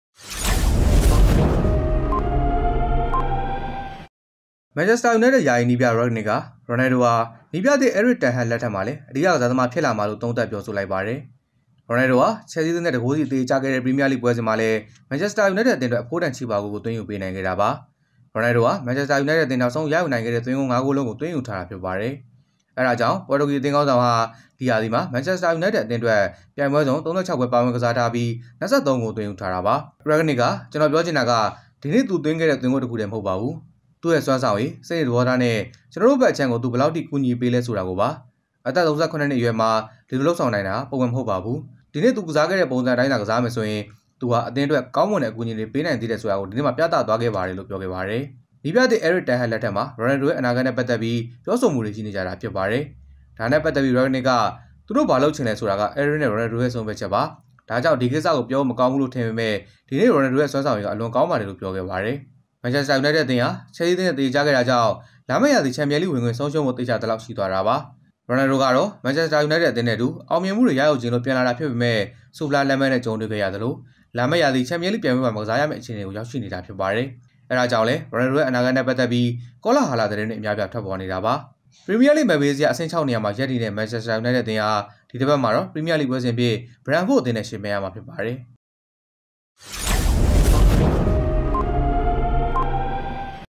ရော်နယ်ဒို က နည်းပြသစ်လက်ထက်မှာလည်း အဓိကဖြစ်လာမှာလို့ ရက်ဂ်နစ် သုံးသပ်
CNI Sport News

၂၀၂၂ ခုနှစ်၊ ဧပြီ ၃၀

မန်ချက်စတာယူနိုက်တက် ယာယီနည်းပြ ရက်ဂ်နစ် က ရော်နယ်ဒို ဟာ နည်းပြသစ် အဲရစ်တန်ဟတ် လက်ထက်မှာလည်း အဓိက ကစားသမားဖြစ်လာမှာလို့ သုံးသပ်ပြောဆိုလိုက်ပါတယ်။

ရော်နယ်ဒို ဟာ ချယ်လ်ဆီး အသင်းနဲ့ ၁ ဂိုးစီ သရေကျခဲ့တဲ့ ပရီးမီးယားလိဂ်ပွဲစဉ်မှာလည်း မန်ချက်စတာ ယူနိုက်တက် အသင်းအတွက် အဖိုးတန်ချေပဂိုးကို သွင်းယူပေးနိုင်ခဲ့တာပါ။ရော်နယ်ဒို ဟာ မန်ချက်စတာ ယူနိုက်တက် အသင်း နောက်ဆုံးရယူနိုင်ခဲ့တဲ့ သွင်းဂိုး(၅)ဂိုးလုံးကို သွင်းယူထားတာဖြစ်ပါတယ်။ အဲဒါကြောင့် ပေါ်တူဂီ အသင်းခေါင်းဆောင်ဟာ ဒီ ရာသီမှာ မန်ချက်စတာယူနိုက်တက် အသင်းအတွက် ပြိုင်ပွဲစုံ(၃၆)ပွဲ ပါဝင် ကစားထားပြီး (၂၃)ဂိုး သွင်းယူထားတာပါ။

Source:TheSun